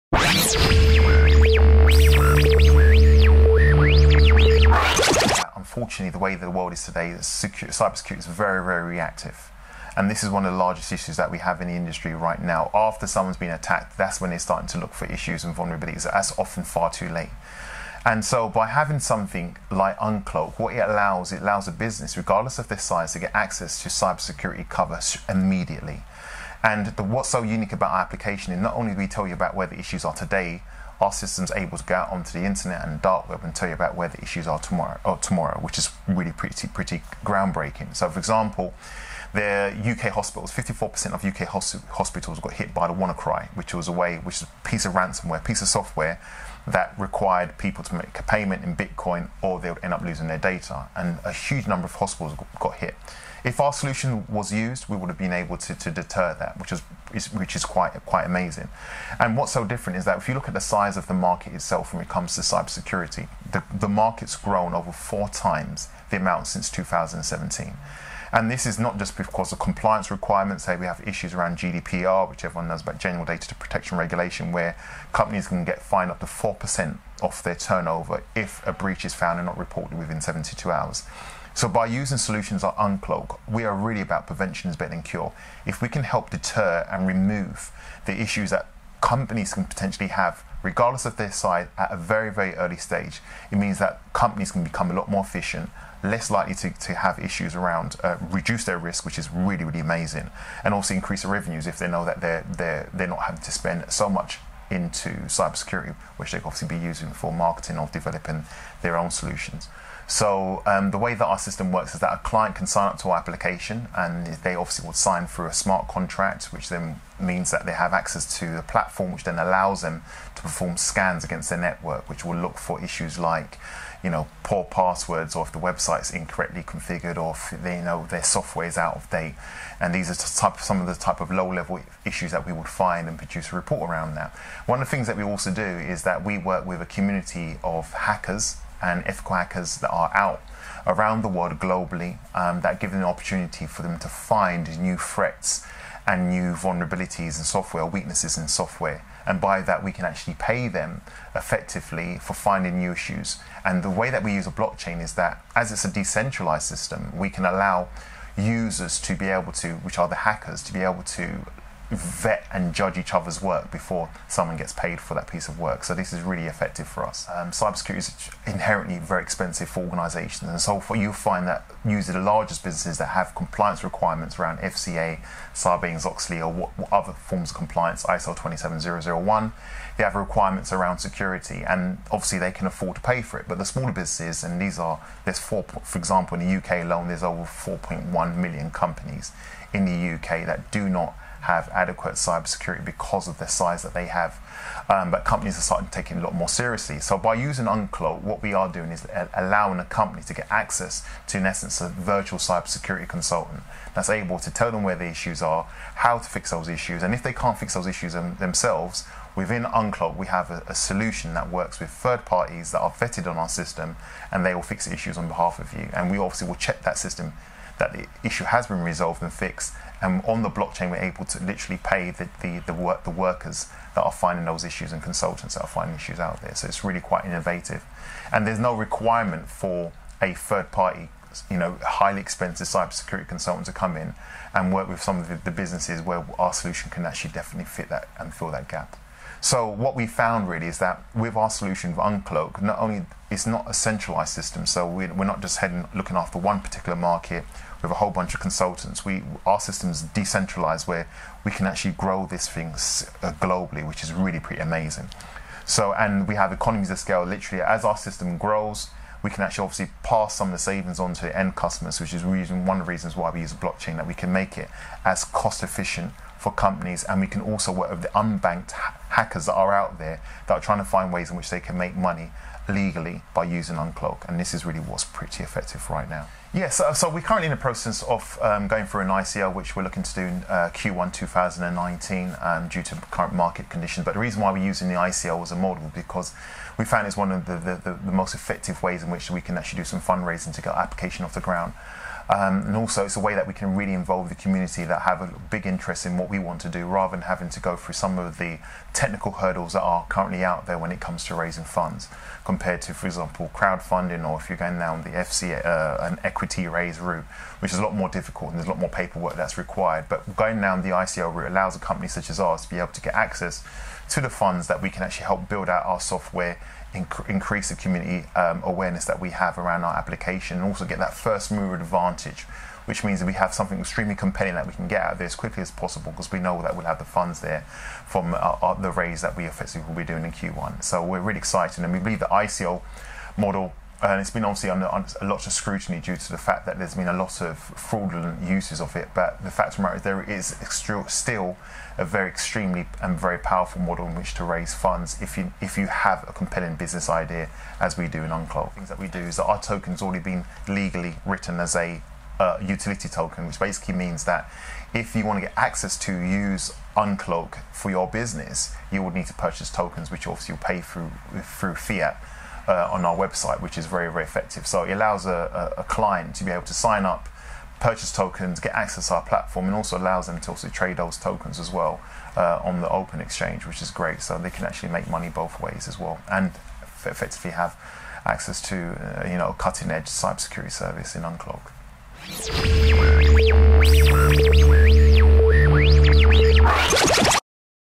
Uncloak – Making cyber security available to all
Tayo Dada, CEO and founder of Uncloak talks about the growing threat to all business of cyber attack and how he has come up with a blockchain based solution that can work for anyone